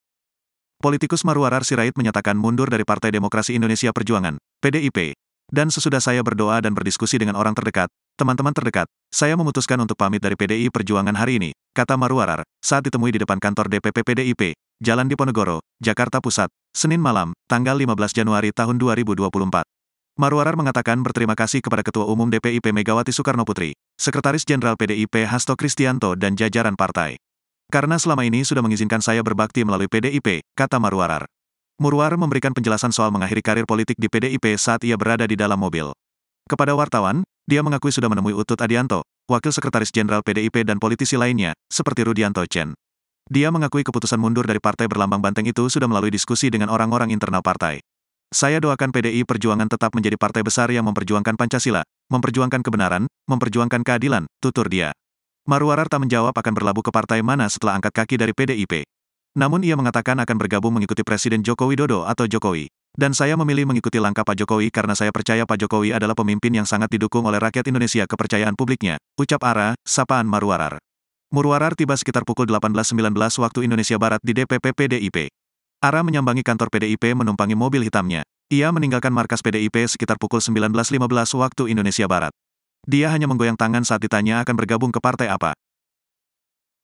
Ada Nama Jokowi Dibalik Keputusan Keluarnya Maruarar Sirait dari PDIP
Sosok yang akrab disapa Ara ini mengatakan, sudah bertemu beberapa elite PDIP untuk berpamitan.
Maruarar tak menjawab akan berlabuh ke partai mana setelah angkat kaki dari PDIP. Namun ia mengatakan akan bergabung mengikuti Presiden Joko Widodo atau Jokowi.